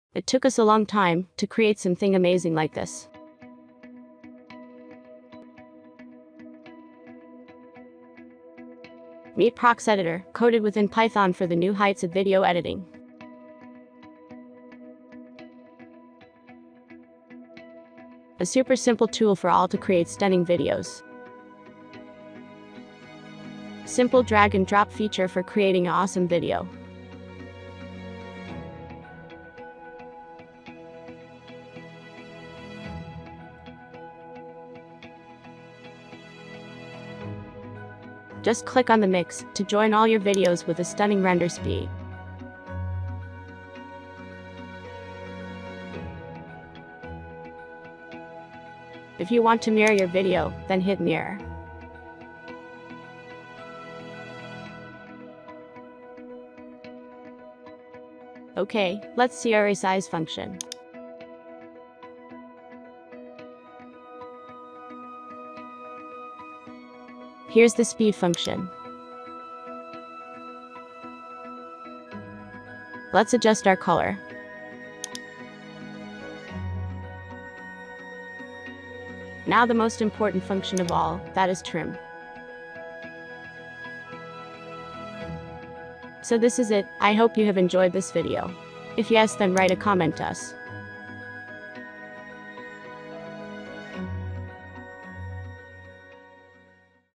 PROX EDITOR PREVIEW | Video editor created using Python and eel
We designed Prox Editor Video Editor to be easy to use, quick to learn, and surprisingly powerful video editor. Take a quick look at some of our most popular features and capabilities.

Features:

Prox Editor is a cross-platform video editor, with support for Linux, Mac, and Windows.

Quickly trim down your videos, and find those perfect moments. It has many easy ways to cut your video.

Control the power of time, reversing, slowing down, and speeding up video. Use a preset or animate the playback speed and direction.

Drag and drop video, audio, or images from your file manager into Prox Editor. It's that easy to get started video editing.

Using our video effects engine, remove the background from your video, invert the colours, adjust brightness, and more.